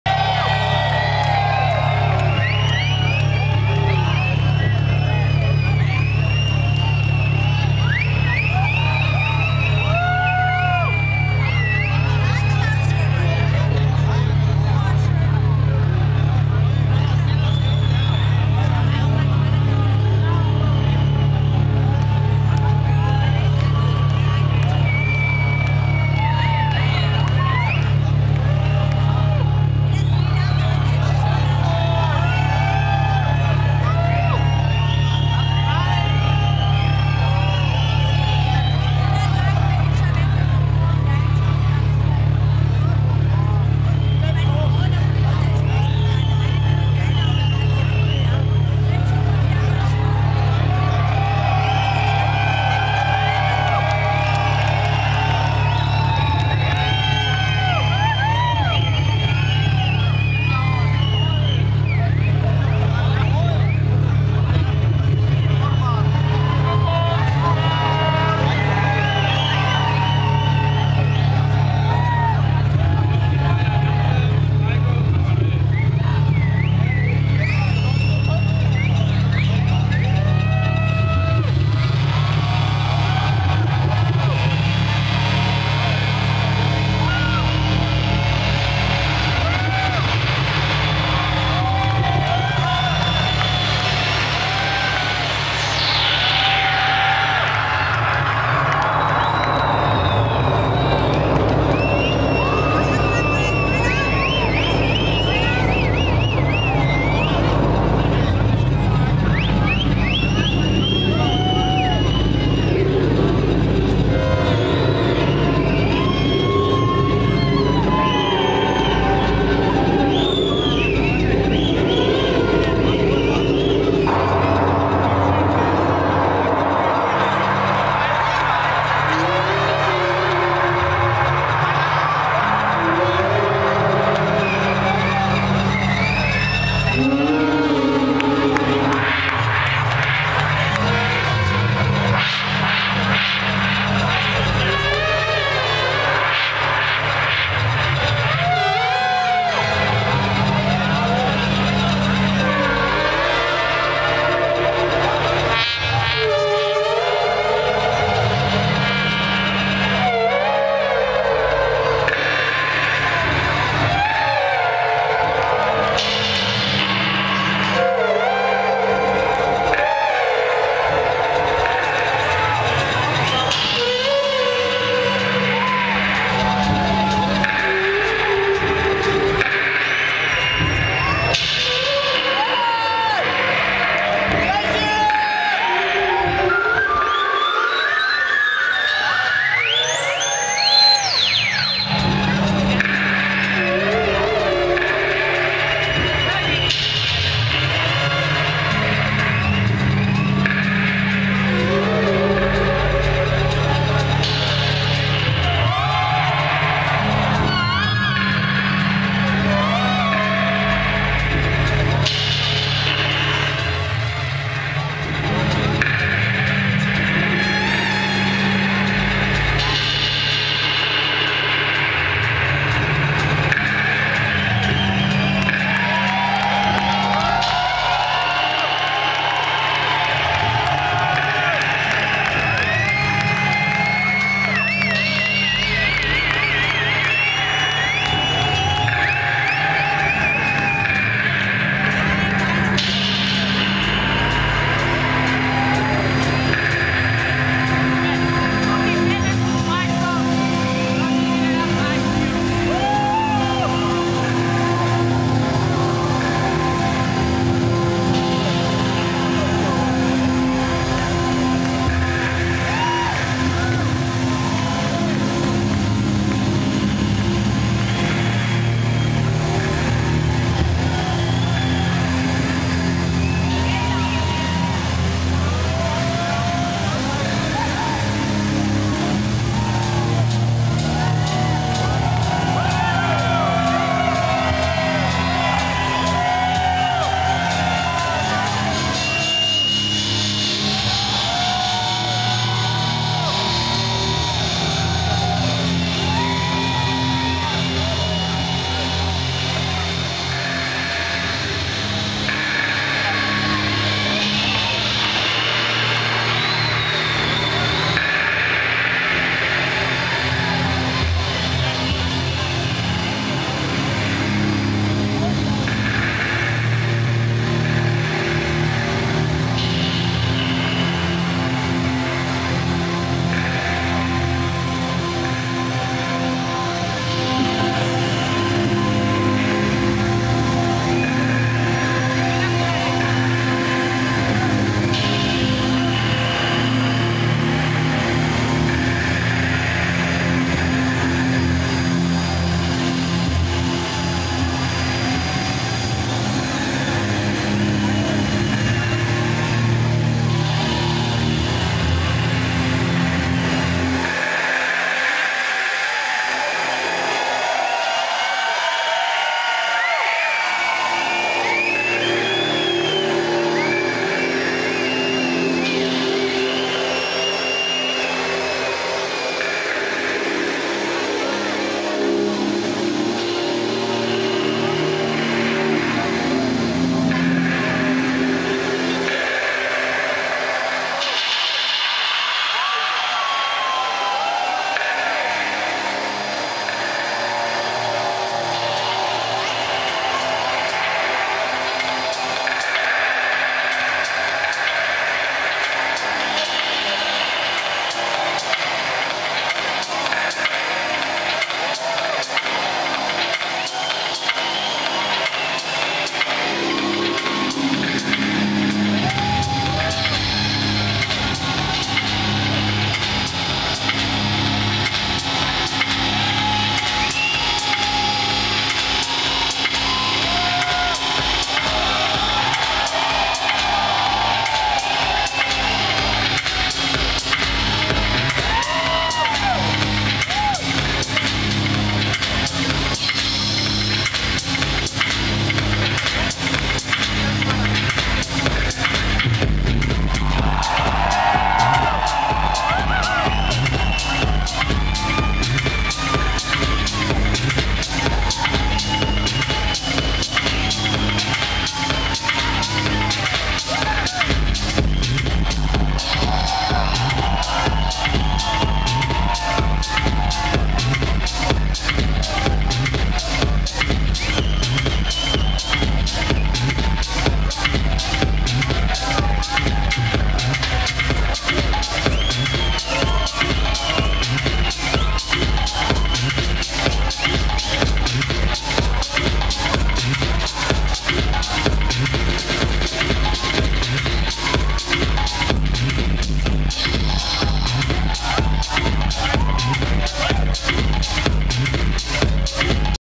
Starting Minus Contakt @ Columbiahalle Berlin 11.10.2009
Apertura del Minus Contakt - 10 anni della M_nus!

In console:

RICHIE HAWTIN, MAGDA & TROY PIERCE
featuring MARC HOULE, HEARTTHROB & GAISER